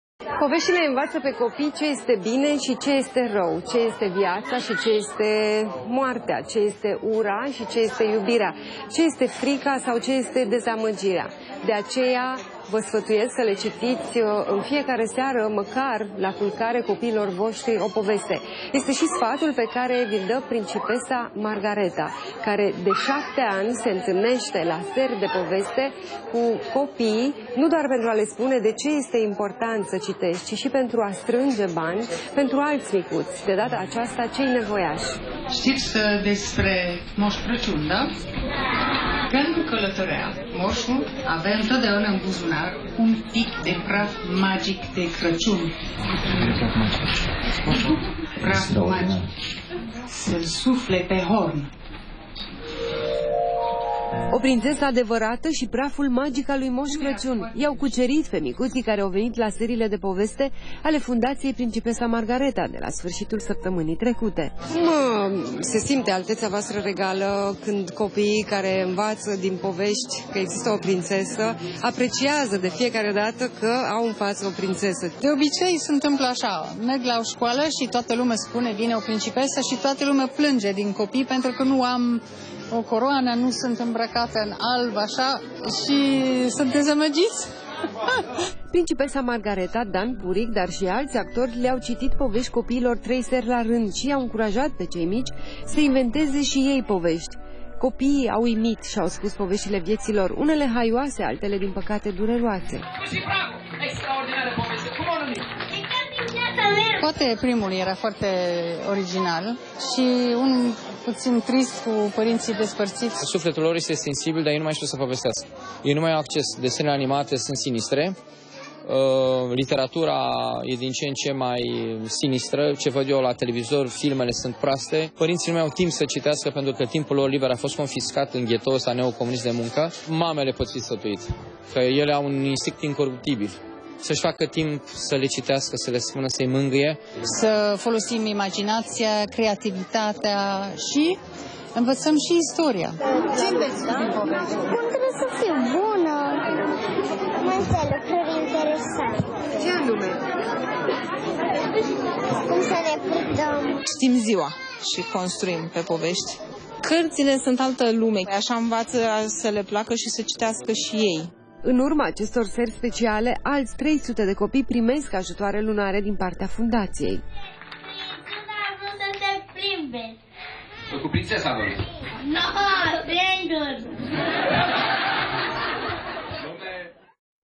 Seri de poveste cu Majestatea Sa Margareta, Librăria Cărturești, decembrie 2013
Reportaj de Camelia Csiki, TVR, 16 decembrie 2013